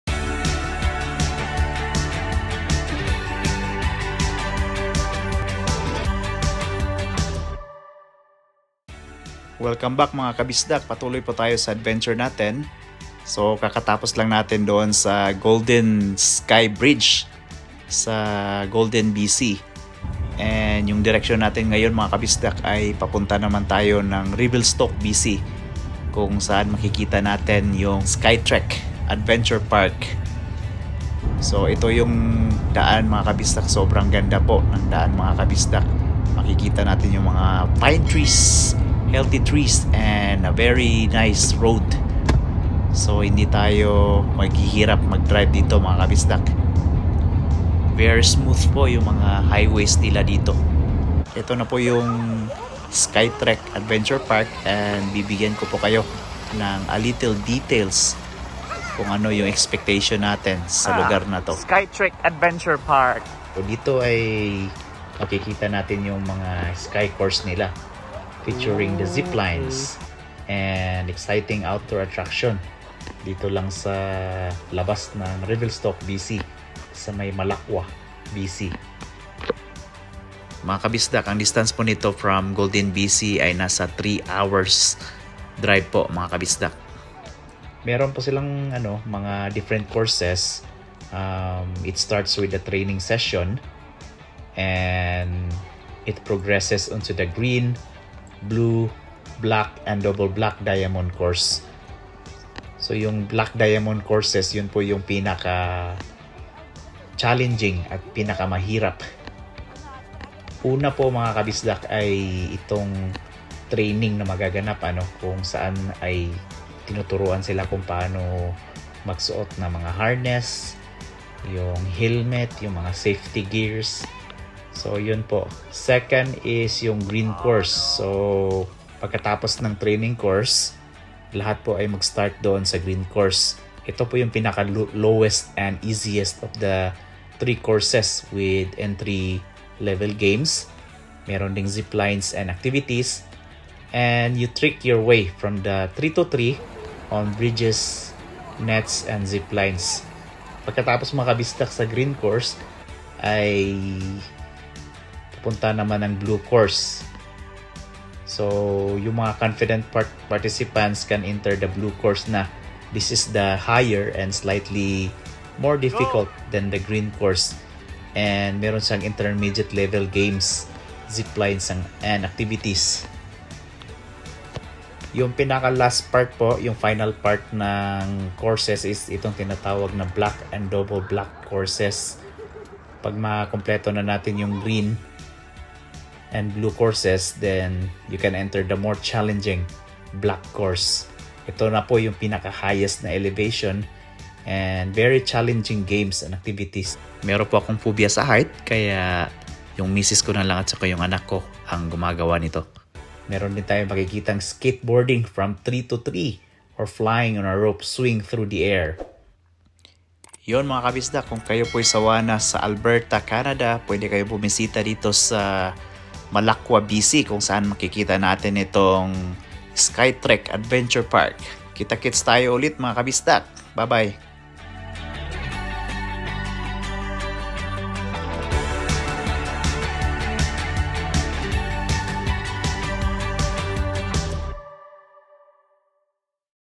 SKYTREK ADVENTURE PARK // MALAKWA BC
Zip line, obstacle course and many more! Family vacation and adventure.